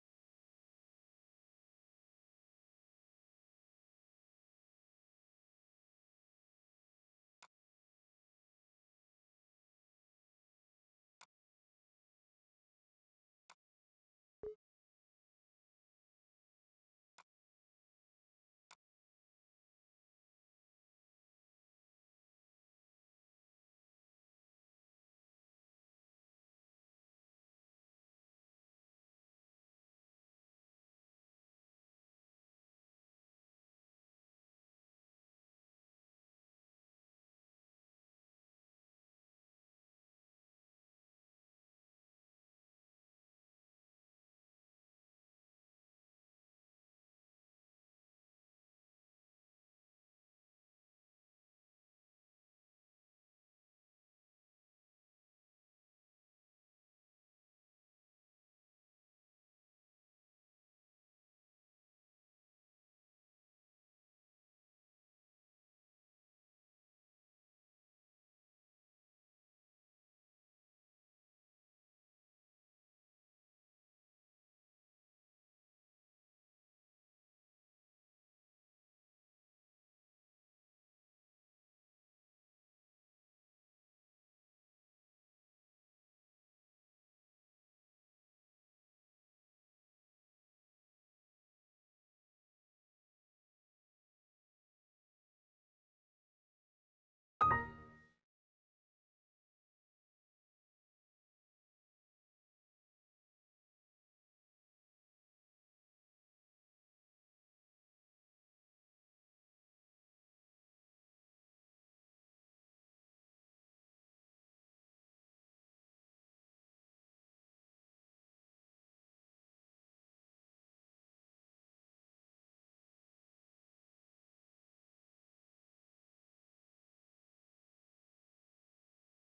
Installing Vmware View Desktop client on Windows XP
Server=remote.warrington.ac.uk - continued below...

A short video showing you how to install the Vmware View Desktop client on Windows XP.

Click on "Online Services"
Then click on "Remote PC Access"

When you install it, please type: "remote.warrington.ac.uk" (without the quotes) as the server name.

After a restart, just login with your normal user name and password and you will have full access to a college workstations from off-site.

Note: After we have restarted the pc in the video, we have blanked out the user name (hence the dark bar that you see)